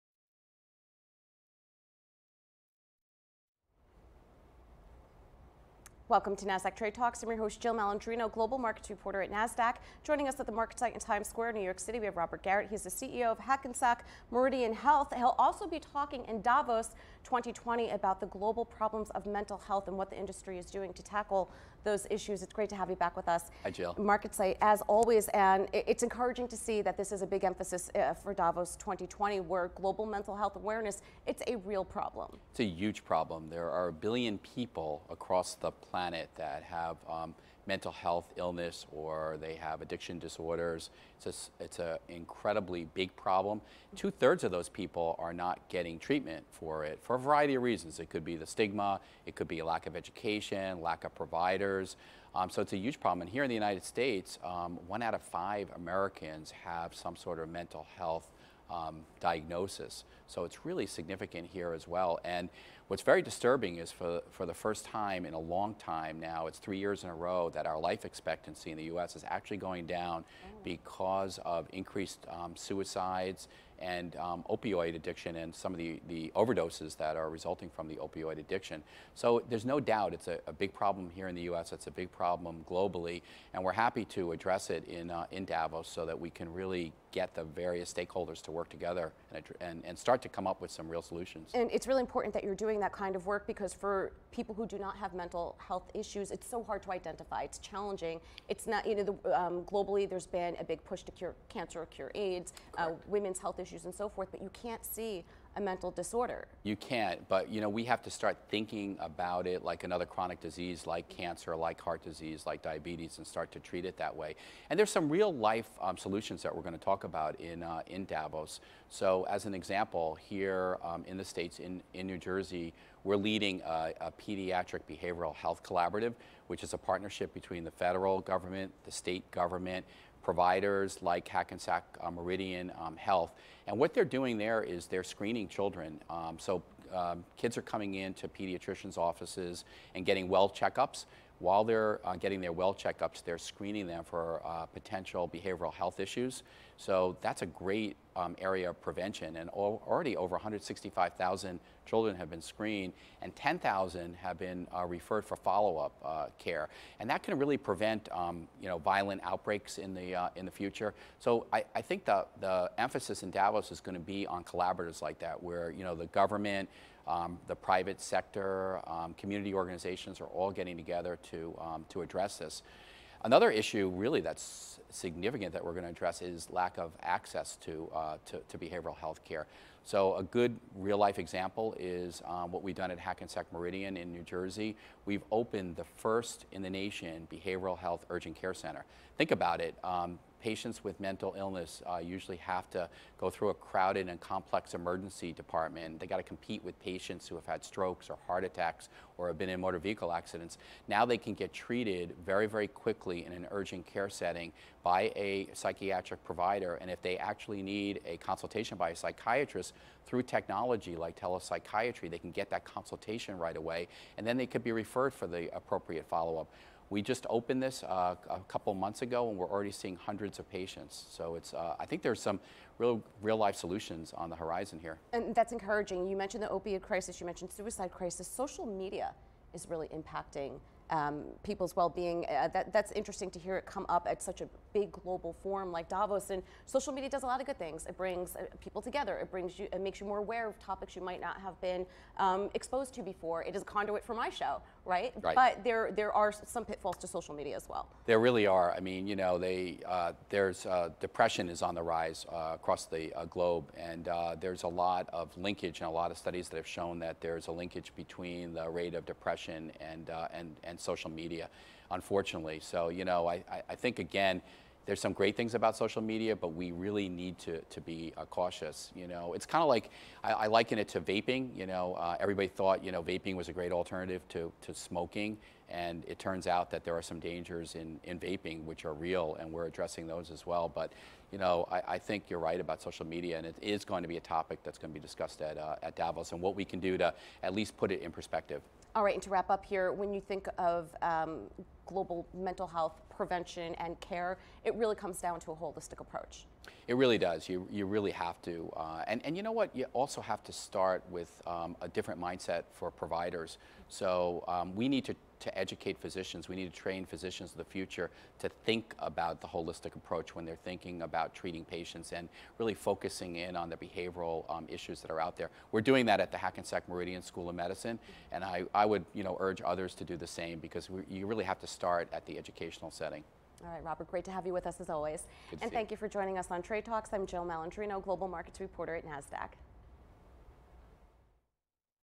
TradeTalks: Moral & Financial Imperatives to Transform Mental Health
Jill Malandrino is joined by Robert Garret, CEO of Hackensack Meridian Health, to discuss mental health and how the Healthcare Industry can Provide Mental Health Solutions.

Follow Trade Talks
Follow our host: Jill Malandrino
Follow NASDAQ
Follow Our Guest: Hackensack Meridian Health